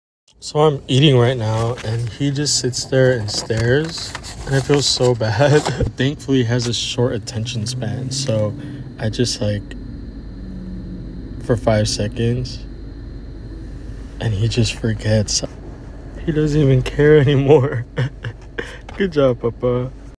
This Rottweiler and Aussie mix gets distracted by everything so fast
This Rottweiler and Aussie mix gets distracted by everything so fast and its so cute